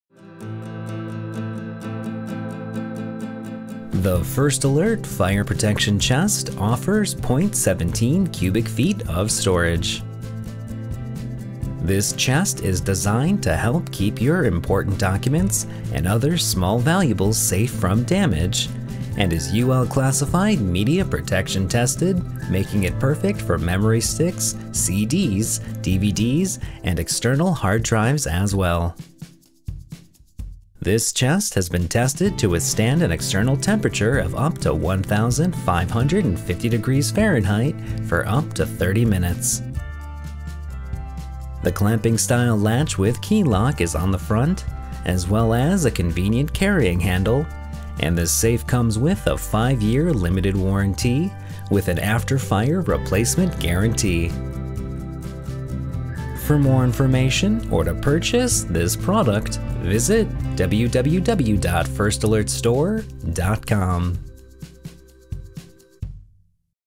First Alert 0.17 Cubic Foot Fire Protector Chest - (2011F)
The First Alert 2011F Fire Chest is designed to help keep your important documents, electronic media, and other small valuables safe from fire damage. This 0.17-cubic foot fire safe has been tested to withstand external temperatures of up to 1550° Fahrenheit for up to 30 minutes.

Independently Tested for Durability

Designed to withstand external temperatures of up to 1550° Fahrenheit for up to 30 minutes, this security safe has been independently verified to protect digital media such as memory sticks, CDs, DVDs, and external hard drives. The document safe helps keep your important documents, jewelry, money, and other small items safe from fire damage.

Keep your items safe from fire at home, in the office, or on the go with the 2011F key safe. This compact safe box has a capacity of 0.17 cubic feet and features a convenient built-in handle for easy portability.

Clamping-Style Lock Secures Your Valuables

The fire-safe box is equipped with a key lock and a clamping-style latch to protect its contents from theft. It includes two keys, so you have a backup in case one is misplaced.